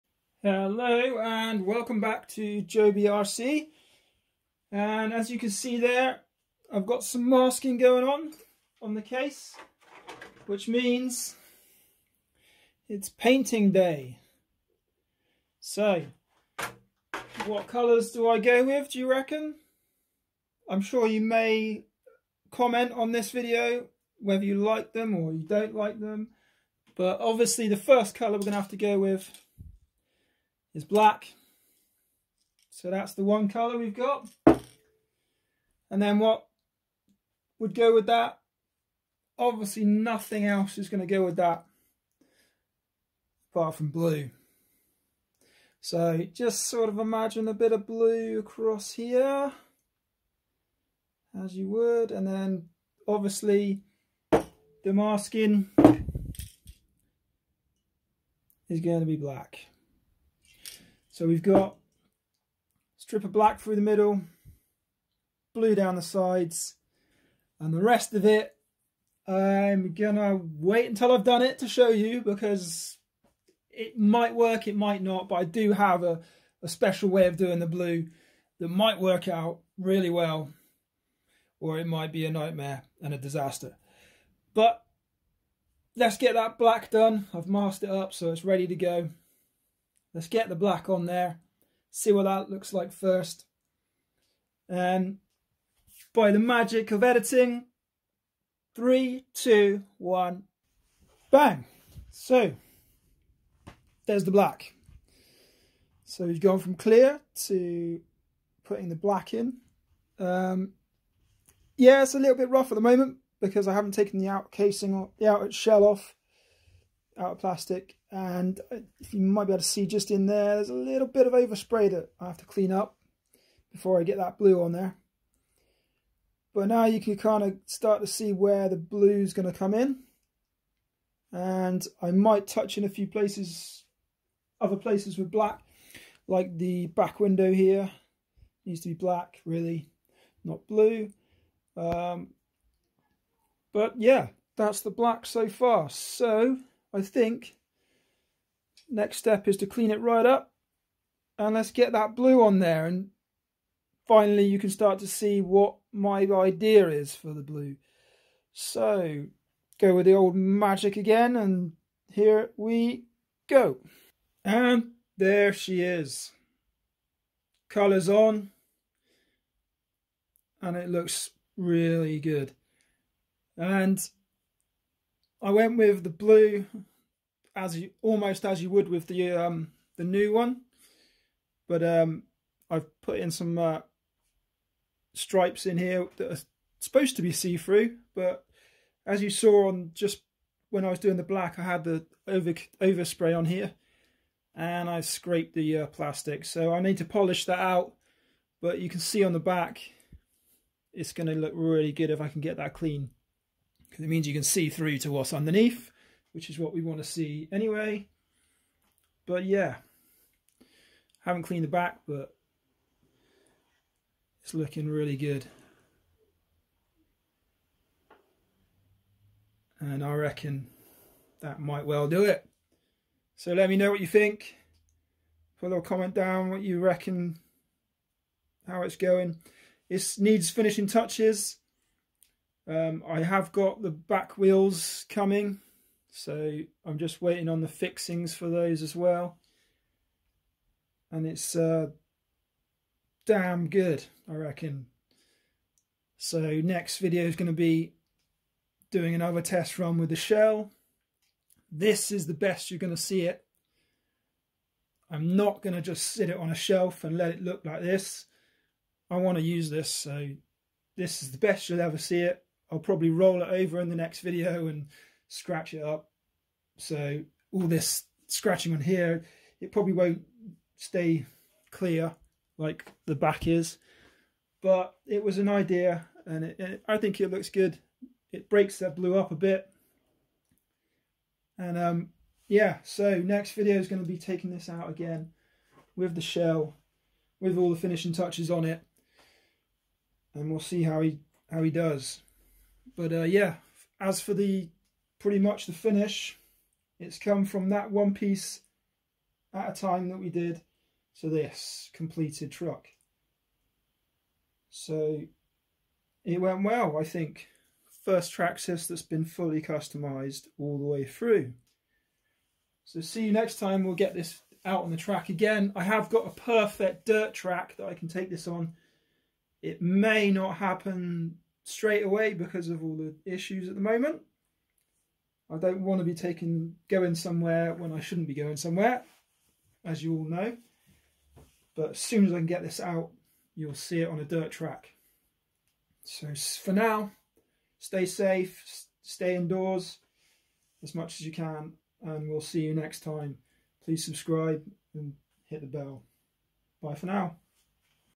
TRAXXAS UDR Build part 19 body and paint
Ok, so just a short video today... but it's finally time to paint the body, and I think you all know what colour I went with... but it does have a bit of a twist to it 🙂 hope you like it! and let me know in the comments what you think?